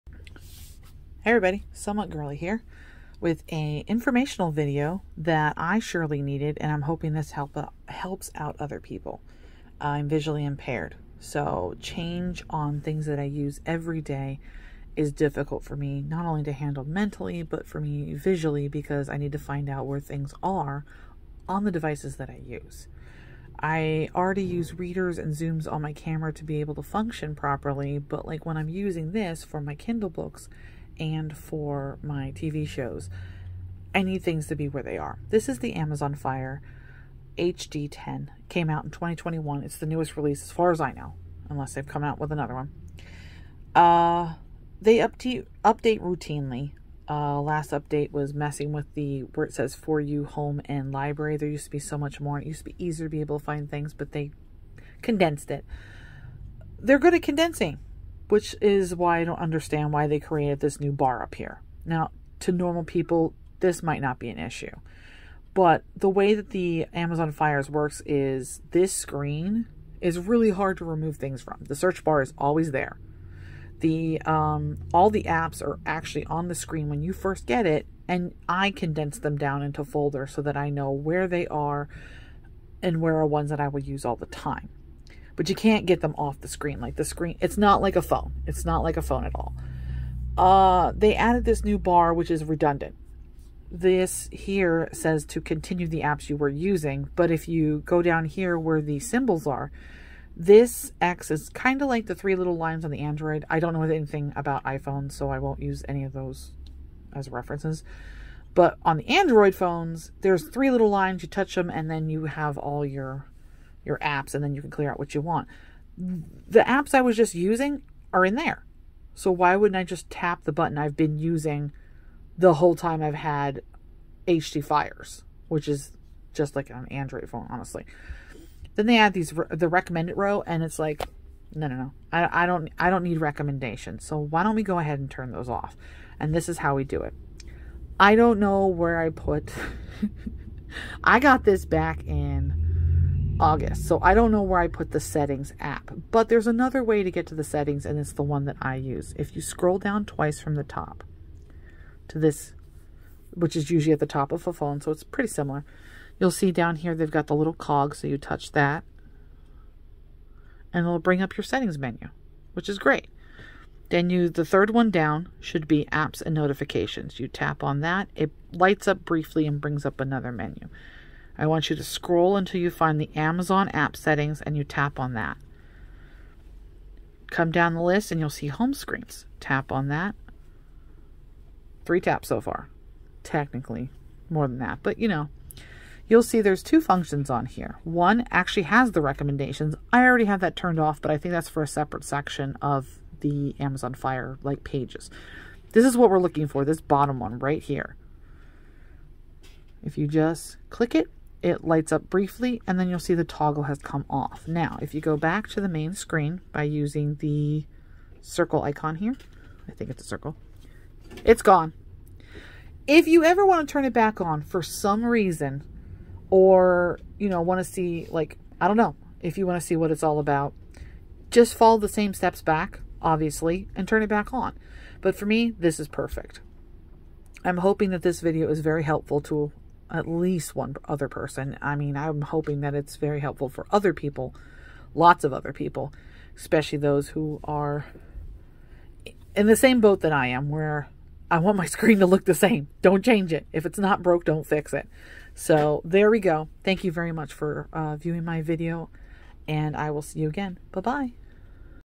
How to turn off the Continue and Recommended Row on Kindle Fire HD - January 2022
The newest update to the HD Fire added something called the Continue and Recommended Row. Want to turn that off? Click my 2nd Time Stamp below to find out!

Wanna get through my babbling? Click on the 2nd time stamp!
Time stamp:
00:00 Start of Video
02:26 How to turn off the Continue and Recommended Row on Kindle Fire HD
03:48 Needed 3 markers to get this to work lol

I own an Amazon Fire HD 10 (11 Gen 2021)  I really hate when electronics get updates and they add things to the screens that maybe we didn't need nor want.

1. This is NOT a paid sponsership. I buy this subscription monthly.

2. I am visually impaired. If something is not accurately described I apologize in advance.